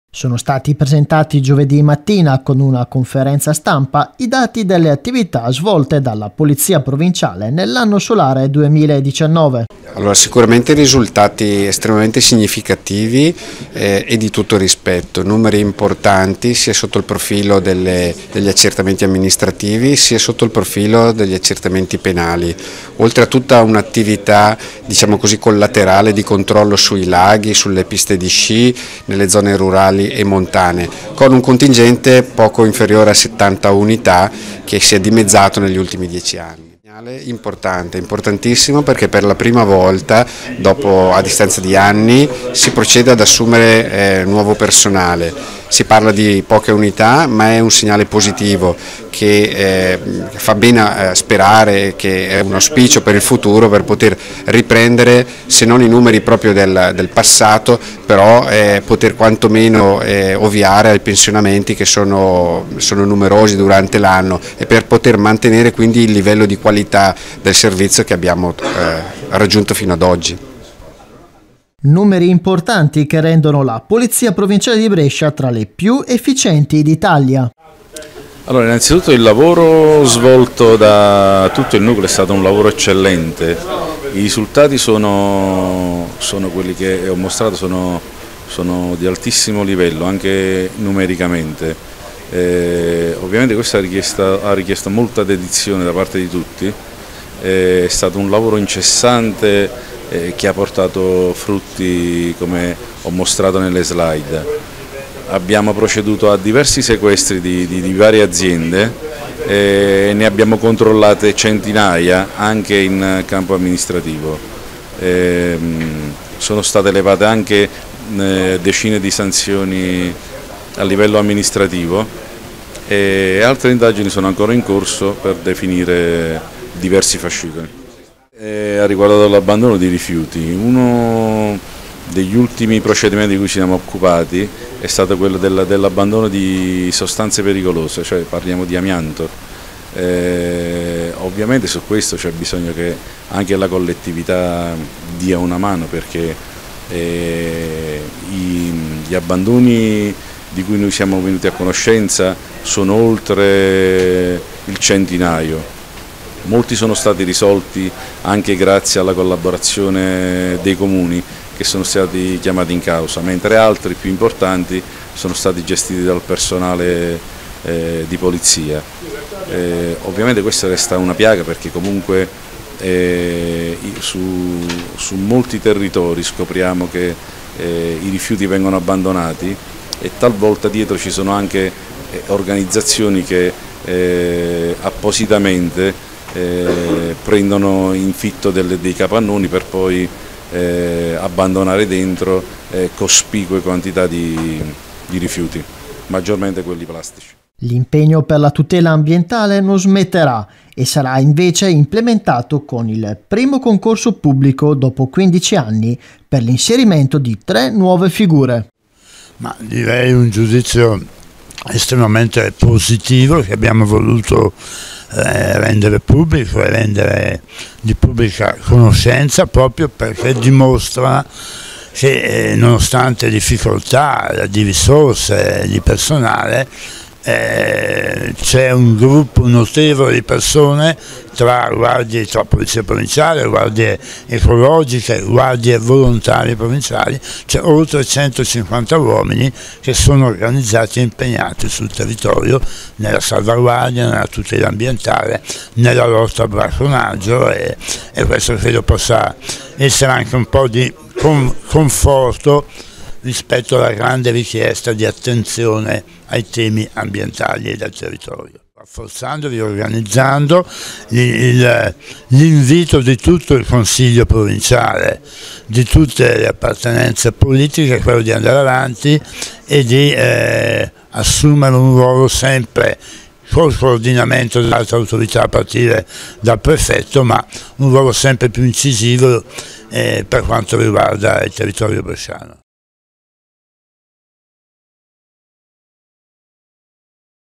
Polizia Provinciale di Brescia: report attività 2019
Presentato il report delle attività svolte nel 2019 dalla Polizia Provinciale della Provincia di Brescia.
Grande lavoro a tutela dell'ambiente e della fauna